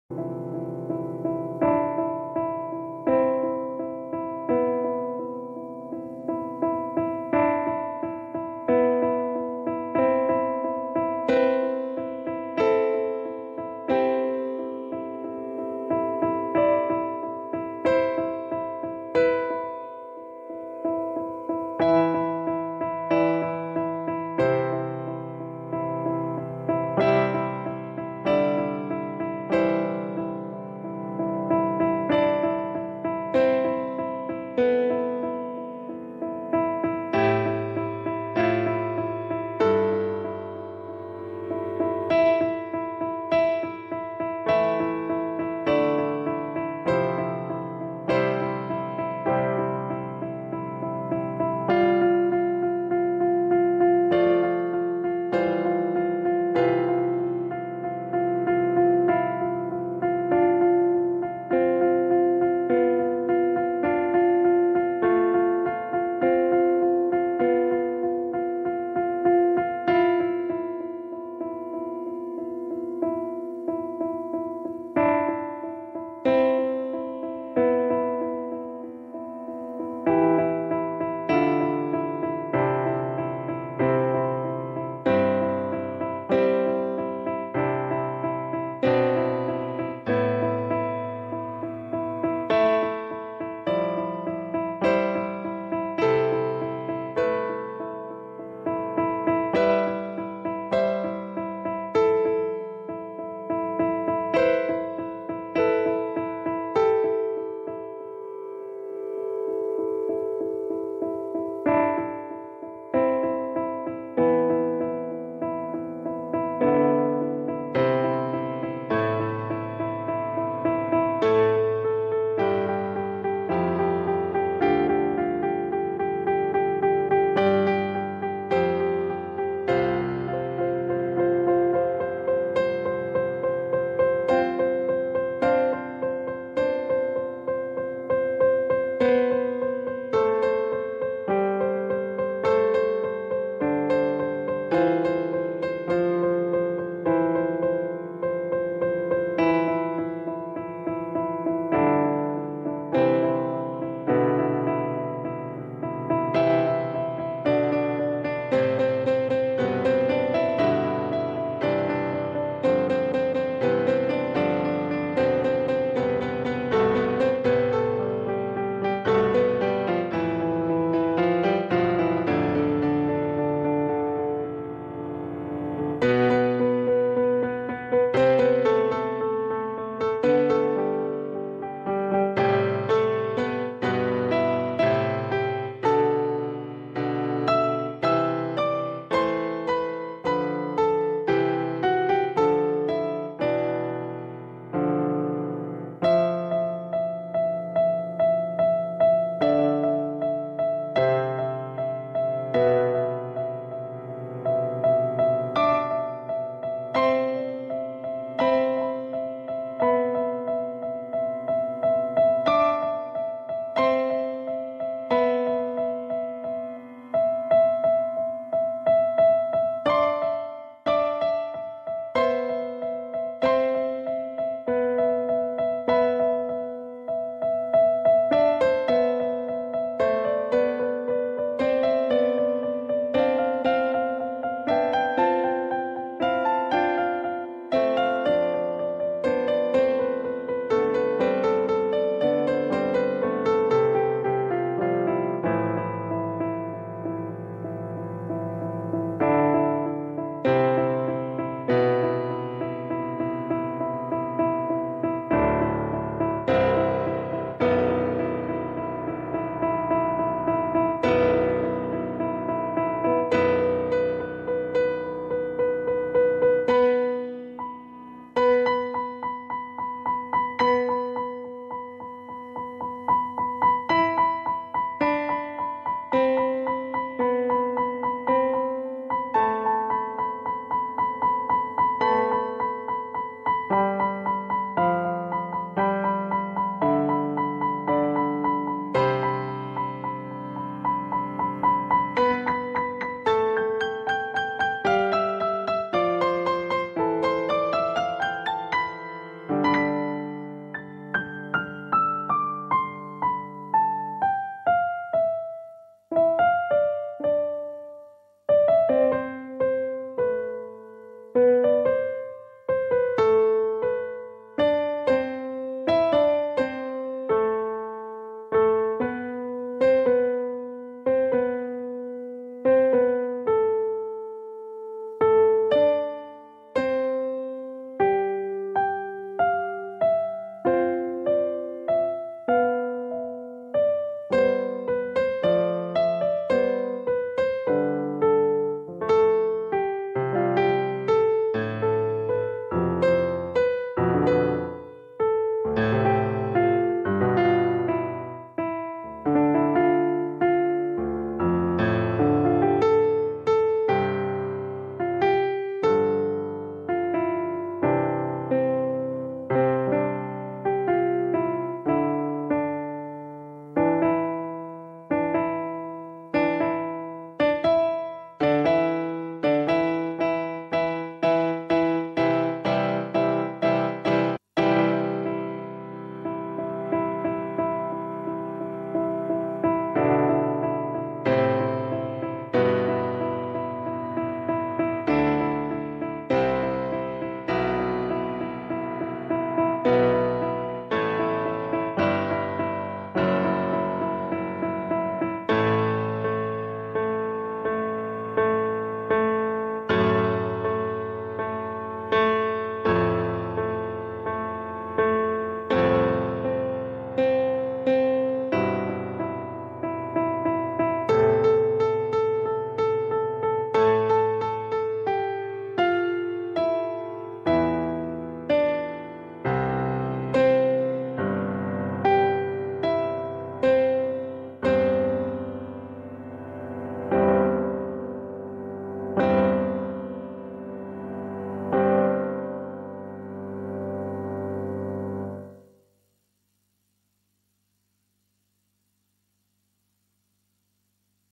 How I Felt During Space X Launch -Improvisation Piano Music-
Orada olmayı gerçekten çok istedim. Bu yüzden hislerimi anlatmaya çalıştım. Müzikteki gerginliğin sebebi bu.

I really wanted to be there so i tried to tell how i felt. The tension in the music was because of that.